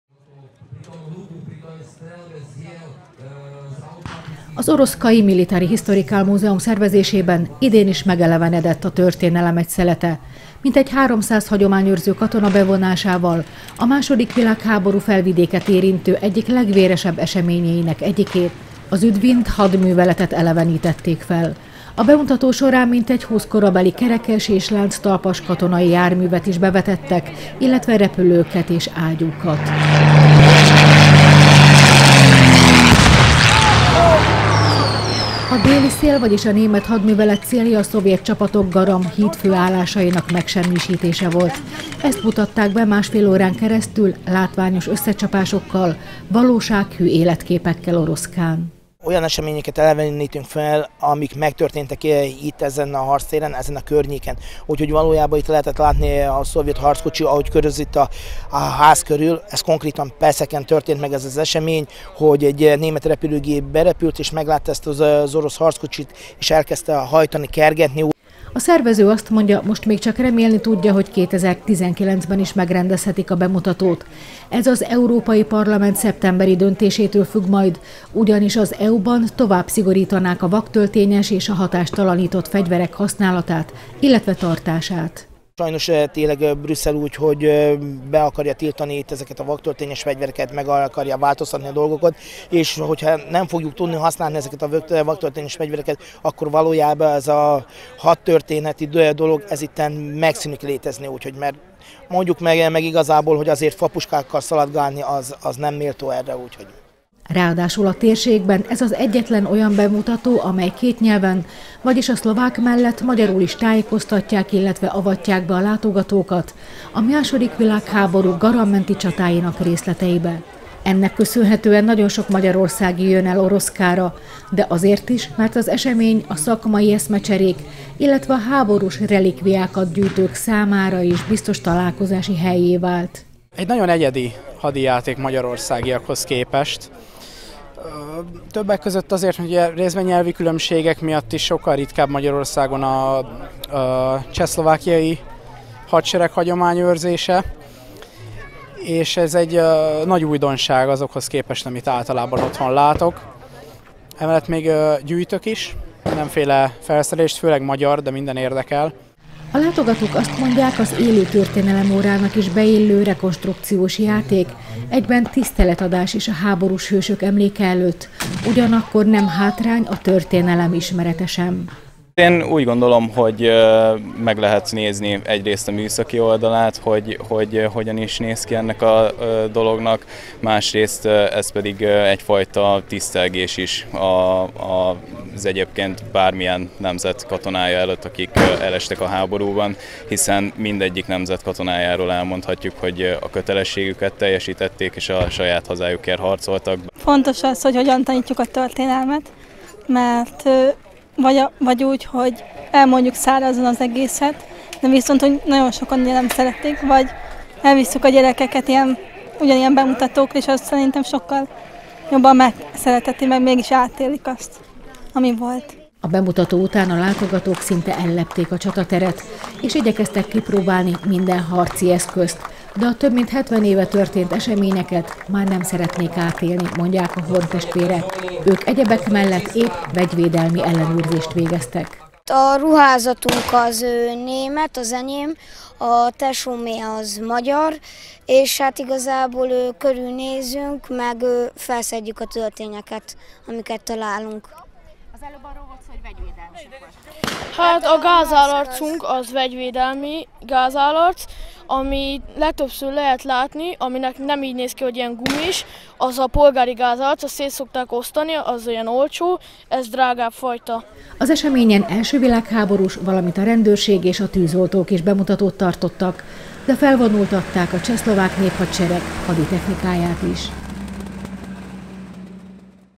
Ismét felelevenítették a Déli szél hadműveletet Oroszkán
Oroszka. Az oroszkai Military Historical Museum szervezésében idén is megelevenedett a történelem egy szelete. Mintegy 300 hagyományőrző katona bevonásával, a második világháború Felvidéket érintő egyik legvéresebb eseményeinek egyikét, a Südwind hadműveletet elevenítették fel.  A bemutató során mintegy 20 korabeli kerekes és lánctalpas katonai járművet is bevettek, illetve repülőket és ágyúkat.